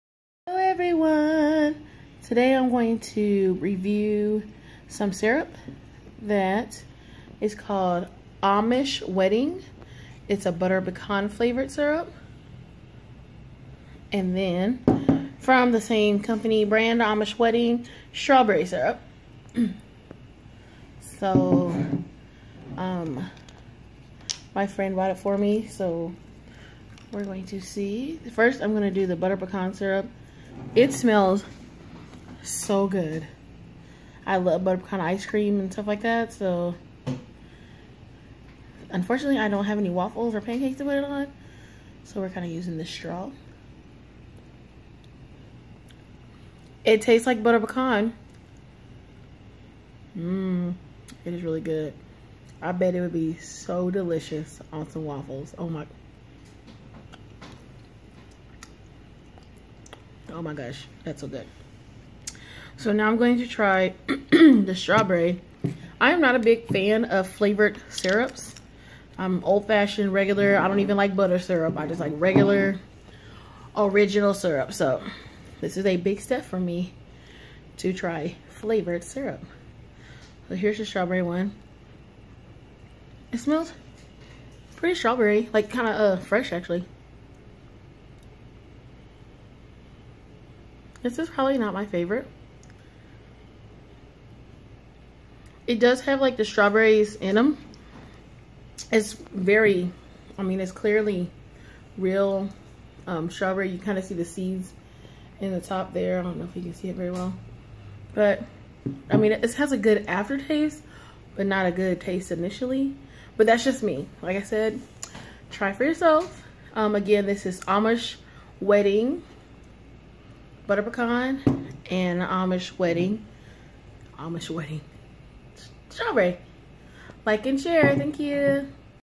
Amish Wedding syrup! Tasting strawberry flavor and Butter Pecan flavor!
Tasting and reviewing these 2 syrups my friend brought me!